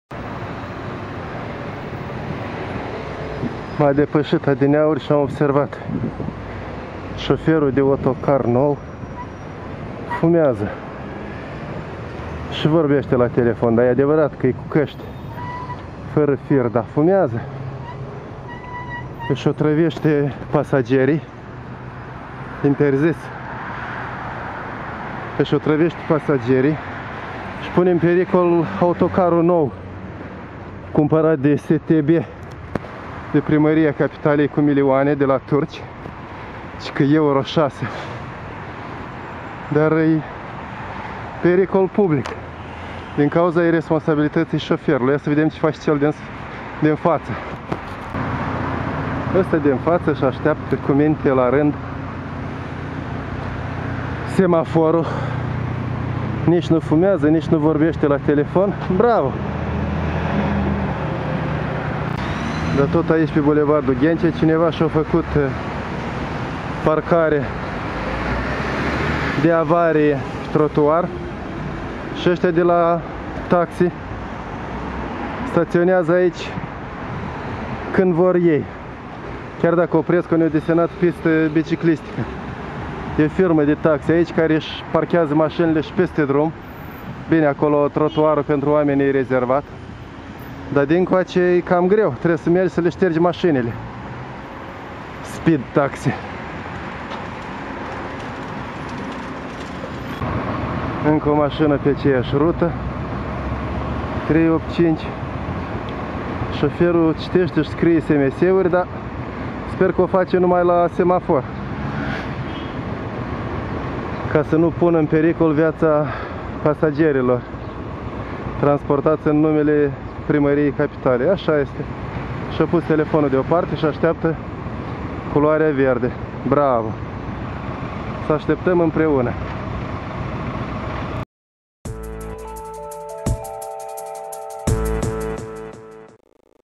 Șofer de Otokar nou, fumează și vorbește la telefon din mers - Curaj.TV
București, bulevardul Ghencea pe lîngă cimitirul cu același nume